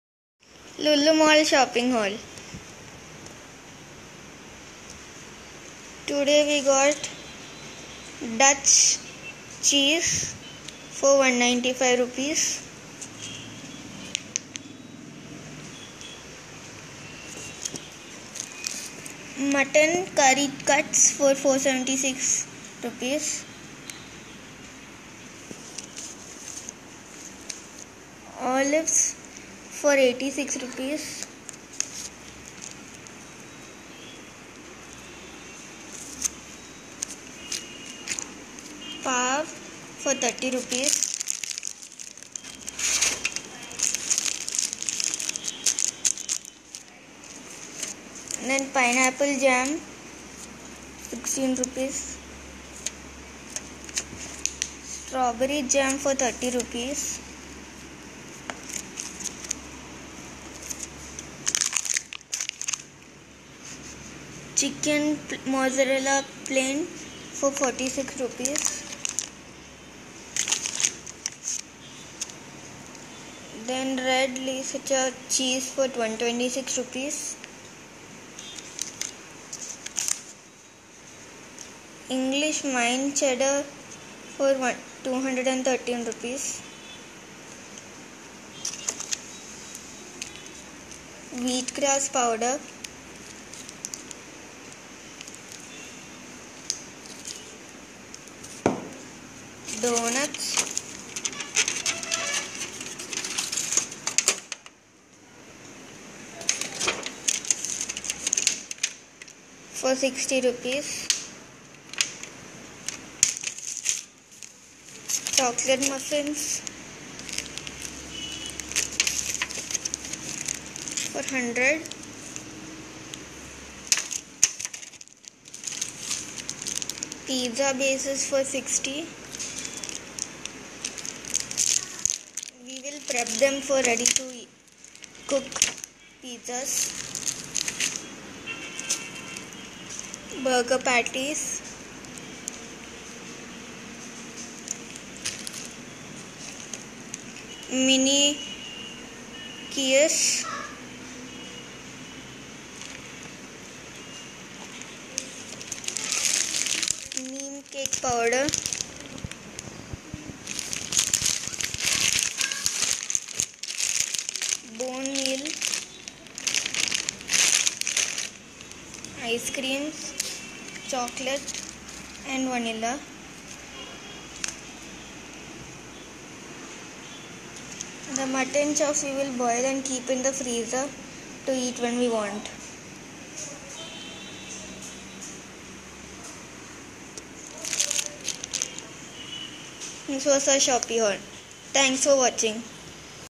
Lulu international shopping mall kochi haul 9 July 2016
Lullu mall shopping haul in lulu international shopping mall located in kochi kerala.